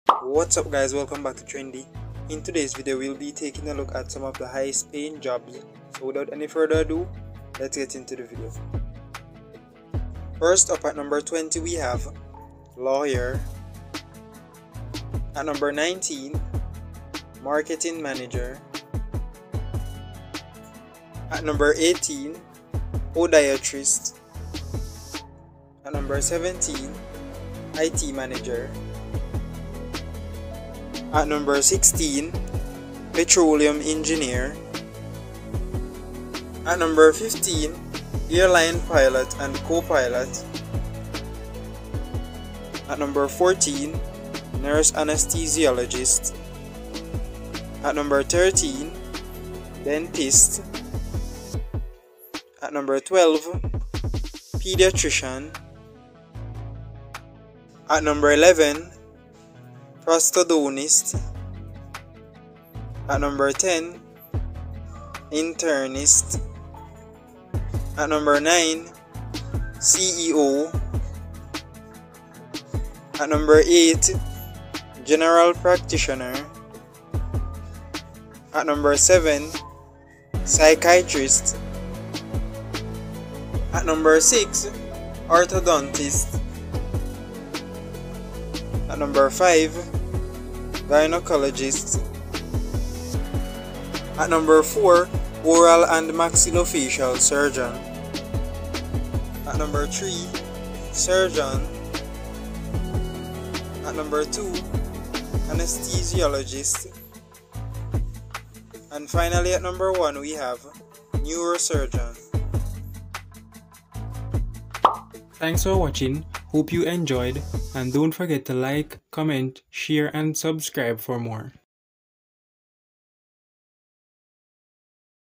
Top 20 Highest Paying Jobs - 2024 🤑
In this video, we're looking at the 20 Highest Paying Jobs. It's hard to choose just 20 Jobs for this list, but we've chosen the jobs with the highest average salaries. From engineering to finance to health care, we'll cover it all. If you're looking for a high paying career path, be sure to check out these top paying jobs. Are you ready to see the 20 Highest Paying Jobs - 2023?

"Highest Paying Jobs - 2023 🤑"

It is not my intent to in any way infringe on their content ownership. If you happen to find your art or images in the video, please let me know and I will be glad to credit you.